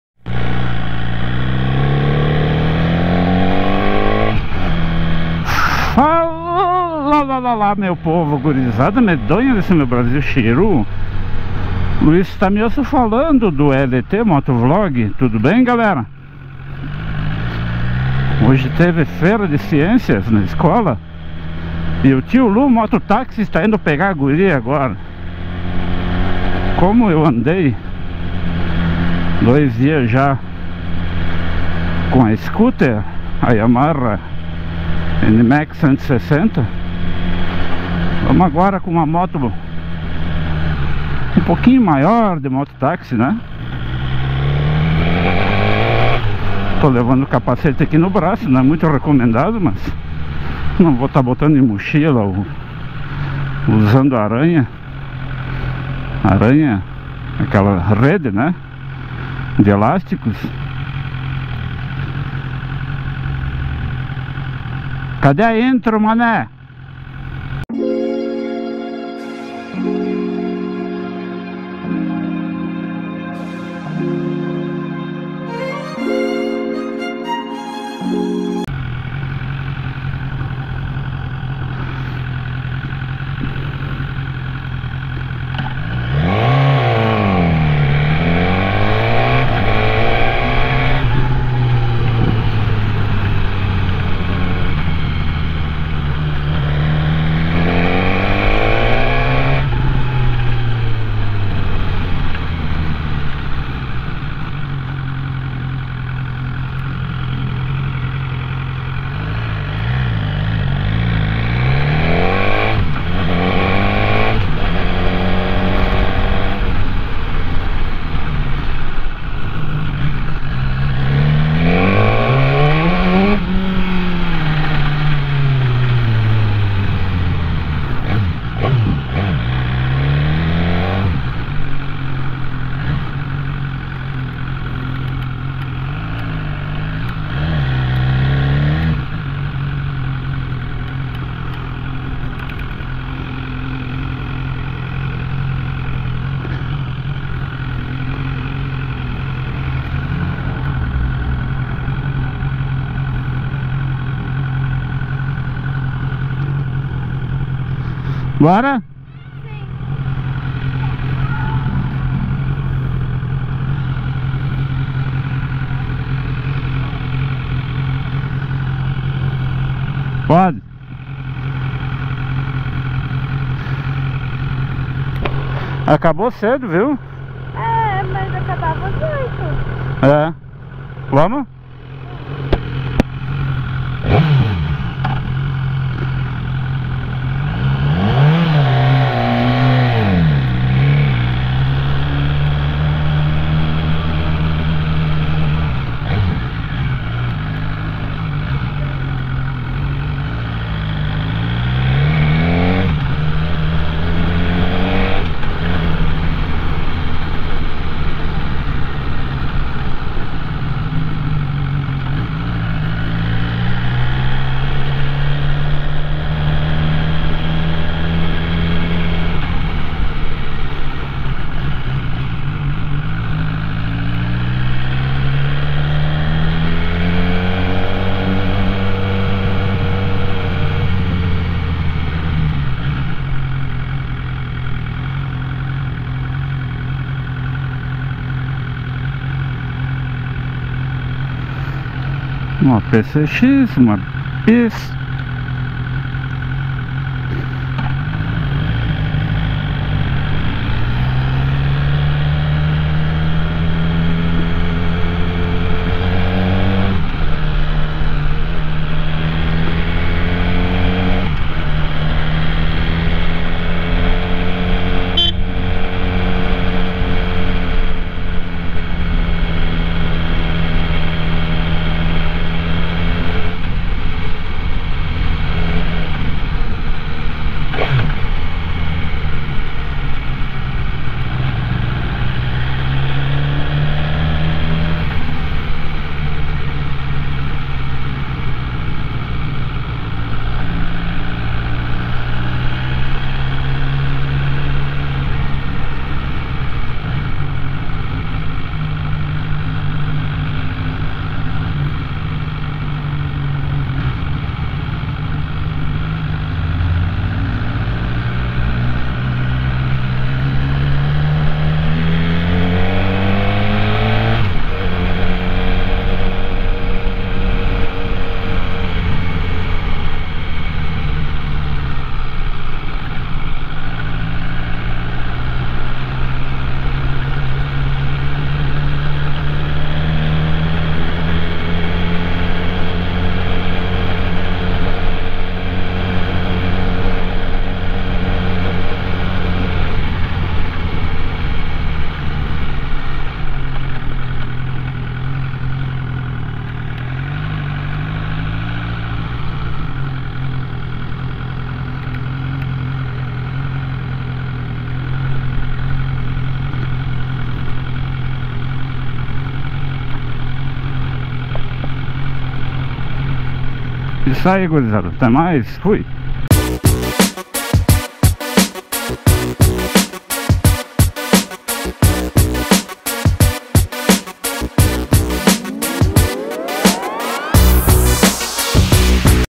De BMW S1000R virei MOTOTAXI de luxo! :)
Desta vez, fomos pegar a JU na saída da escola. Vamos juntos?

From BMW S1000R I will come MOTORCYCLE TAXI DRIVER luxury!
This time we went to pick up JU on the way out of school. Let's go together?

Capacete Shark Speed-R Carbon Skyn DWK
GoPro Hero6 Black & GoPro Hero4 Silver & Mic by Dudacell
BMW S1000R OU Yamaha NMax 160 ABS
CyberLink PowerDirector16 e AcdSee Ultimate8

NÃO AUTORIZAMOS O USO, REPRODUÇÃO PARCIAL OU TOTAL
DE QUALQUER VÍDEO DO CANAL, SEM PRÉVIA AUTORIZAÇÃO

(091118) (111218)